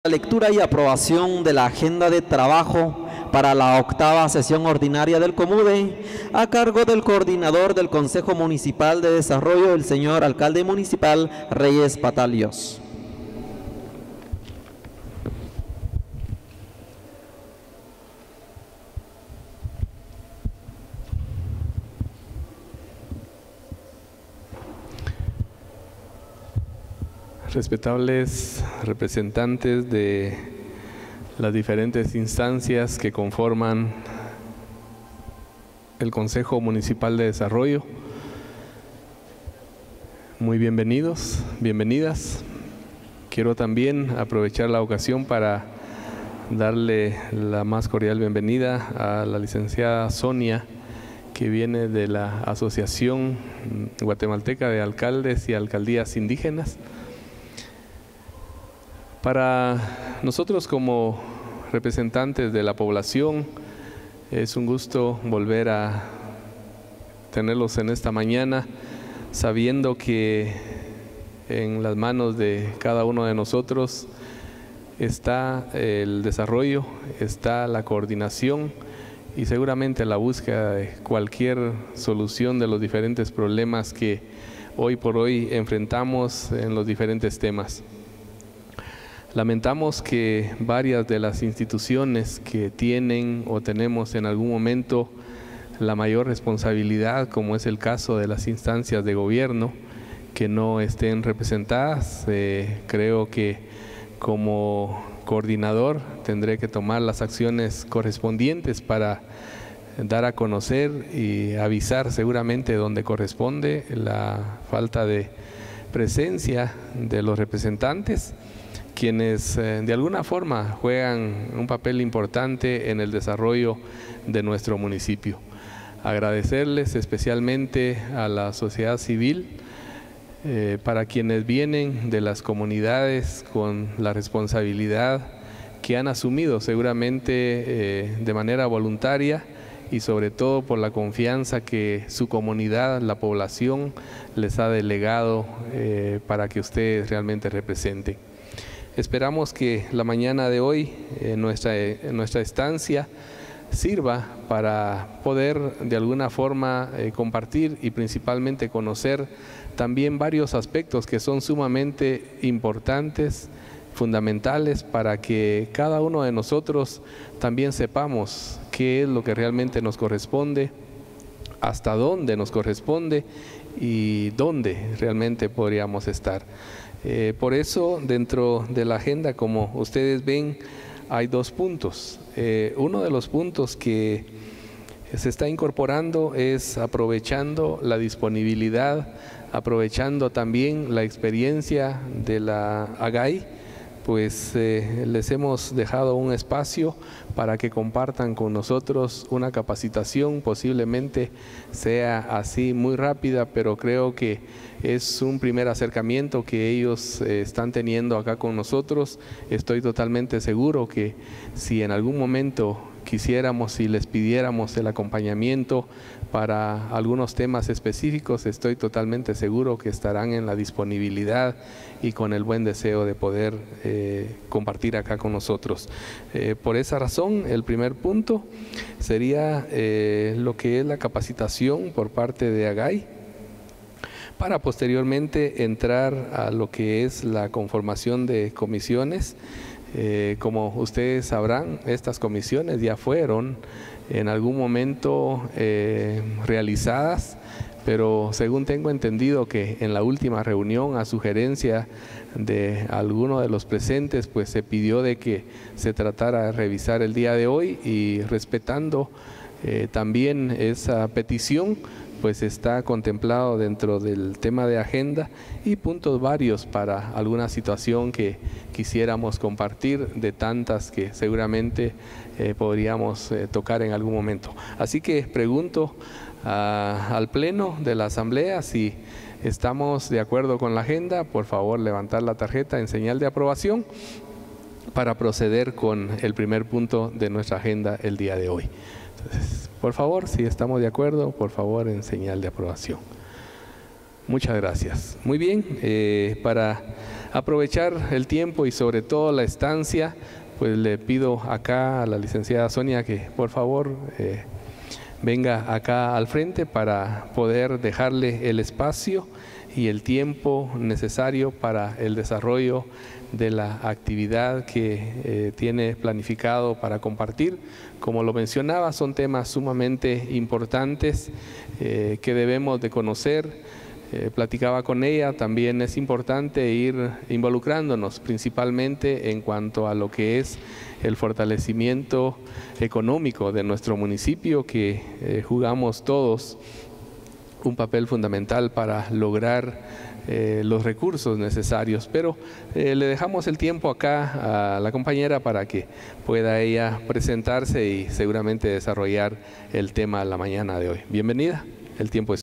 Agenda del COMUDE agosto de 2016
Patzún, lectura y aprobación de la agenda de la reunión ordinaria del Concejo Municipal de Desarrollo, COMUDE, a cargo del Alcalde Municipal Reyes Patal Yos.